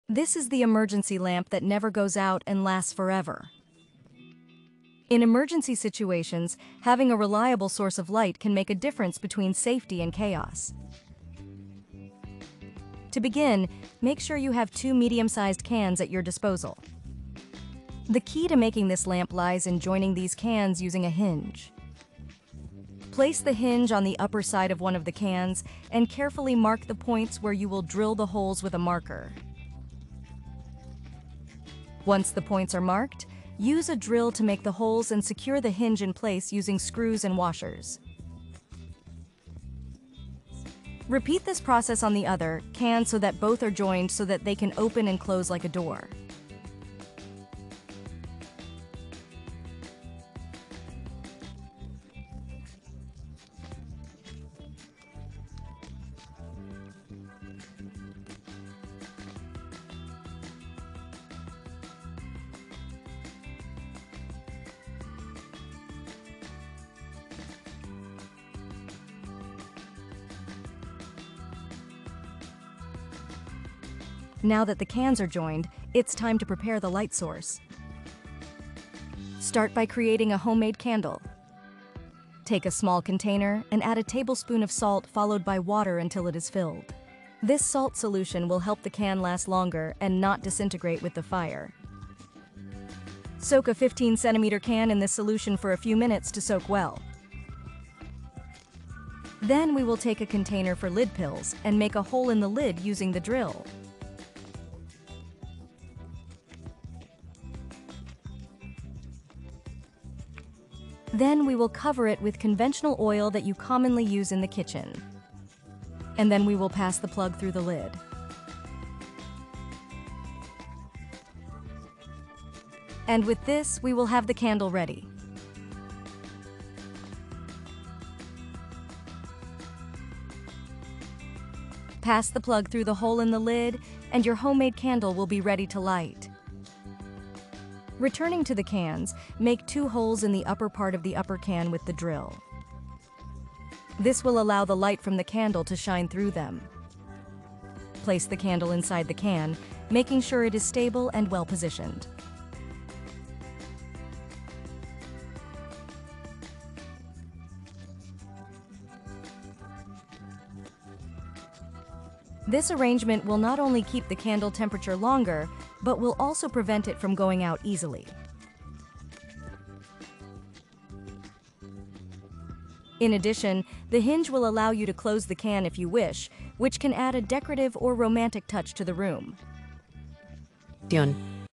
Never-Ending Emergency Lamp: DIY Guide for Infinite Light!
Description: "In this DIY tutorial, learn how to create an emergency lamp that never goes out, ensuring a reliable light source in critical situations. Using just two medium-sized cans and household items, you'll craft a long-lasting solution for emergencies. Follow step-by-step instructions, from joining the cans with a hinge to preparing a homemade candle with a special salt solution for extended durability. Discover how simple materials can provide infinite light when you need it most. Watch now and be prepared for any emergency!"